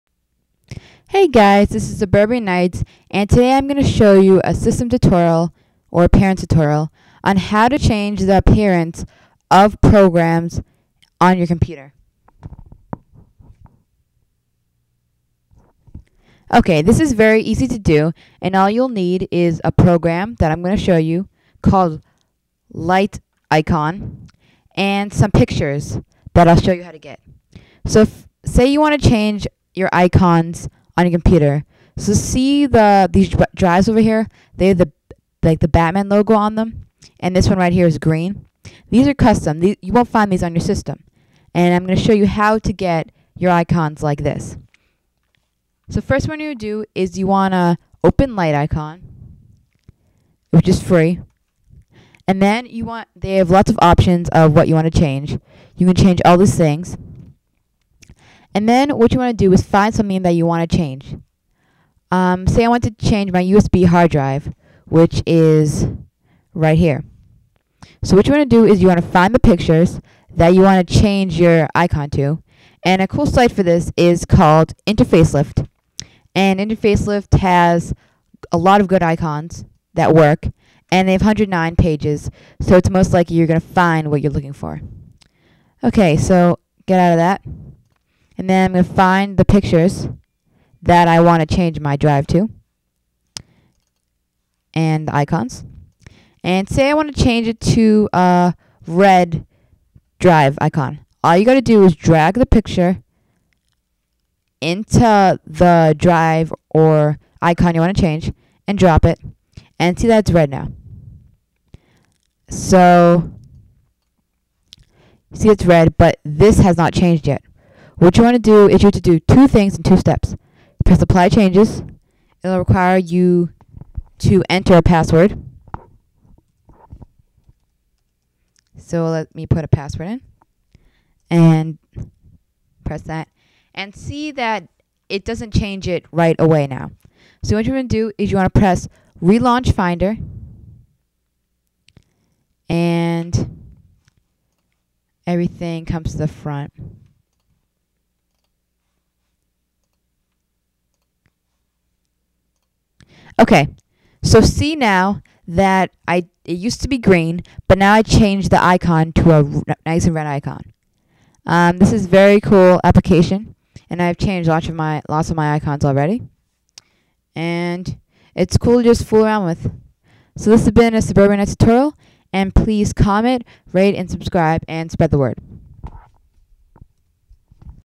How to Change Your Mac Icons
Tired of those boring old default icons for your Mac? Wondering how to change them? Give you Mac some new life!